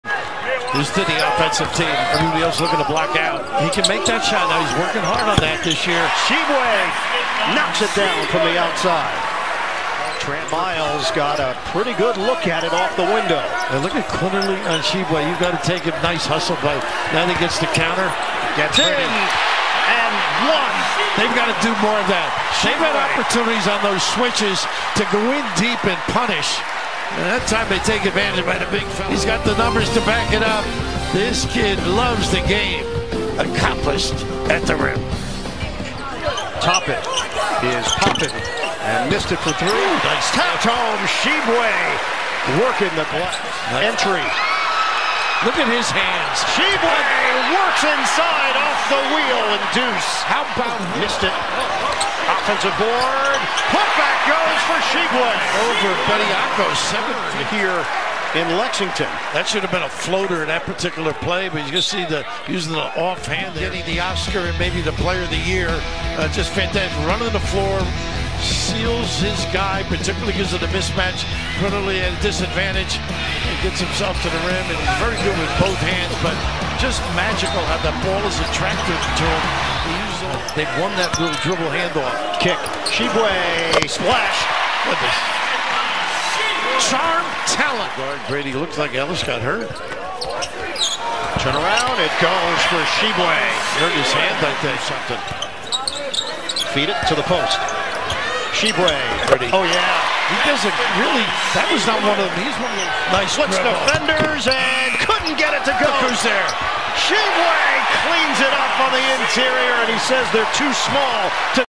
Oscar Tshiebwe Highlights vs #25 Alabama (2/19/2022)
Stat-Line: 9/13 FG, 21 PTS, 14 REB, 3 AST